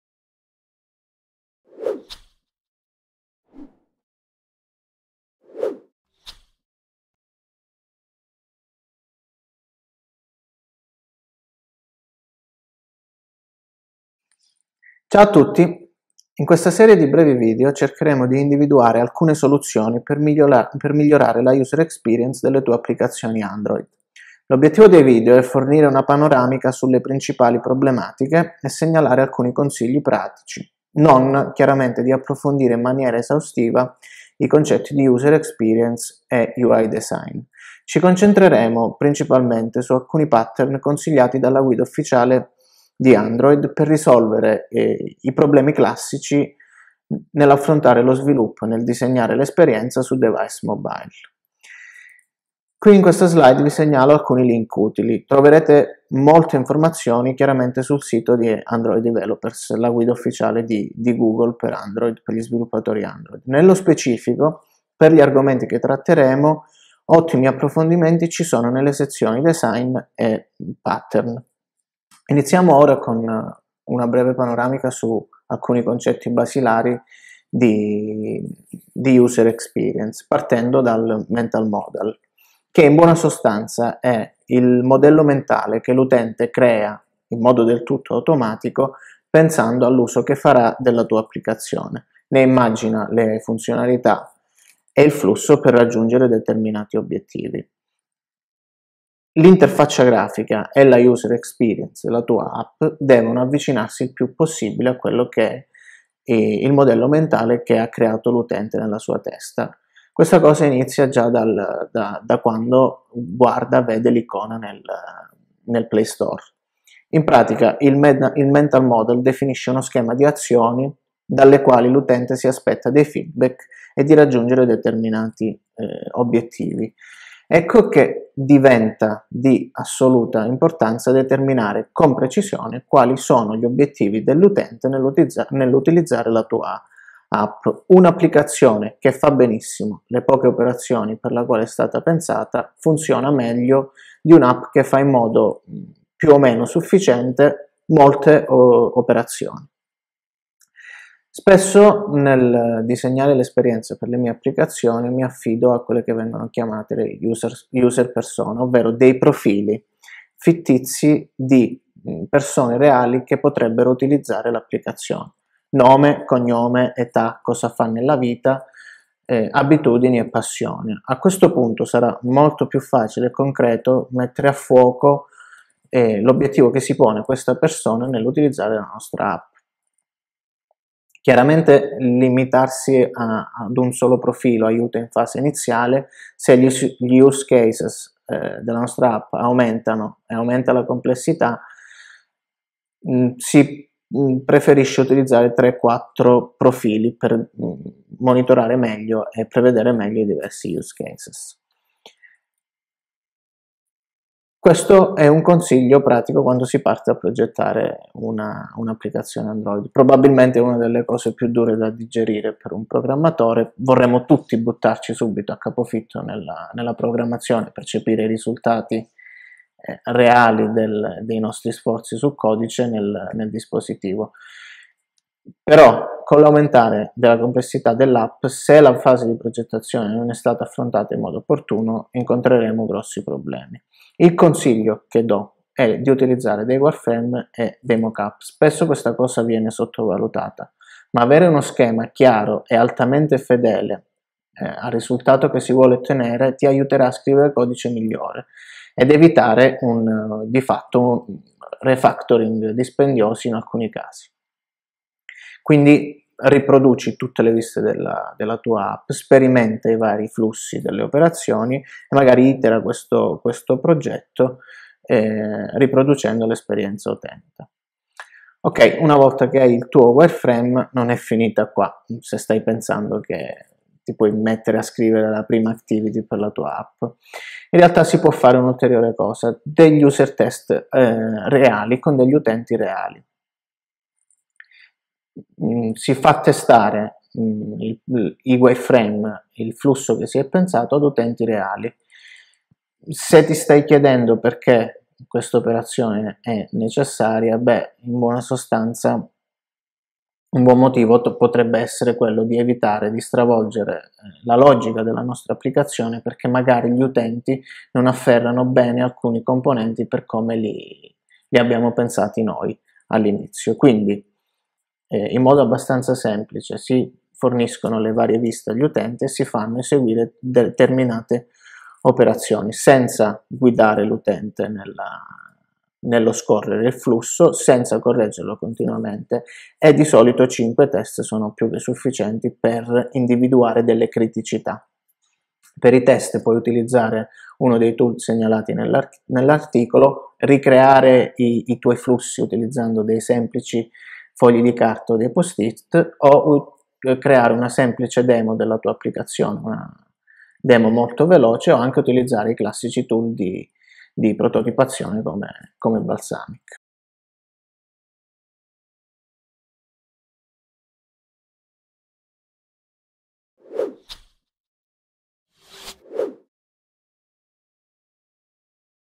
GDL Italia - Android User eXperience (parte 1)
Piervincenzo Madeo ci presenta una nuova serie della GDL Italia, Android User eXperience. In questo primo episodio verranno introdotti mental model, wireframe e user test.

Tutti i video della Google Developers Live Italia sono raccolti in una playlist